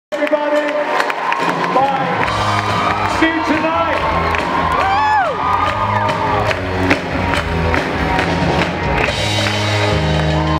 Julian Richings Clip 8 SPN Vegas Con 2017 - Day 2
Clip 8 of Julian Richings (Death)'s panel, at Supernatural Vegas Con 2017, Day 2.

Please excuse any jittering, or moving of the camera. I was having to shoot from far away, between or over people's heads. So I often had to move the camera when people in rows in front of me got in the way.

**THIS MAY NOT BE REUPLOADED, REPOSTED, EDITED, OR GIFFED WITHOUT PRIOR PERMISSION**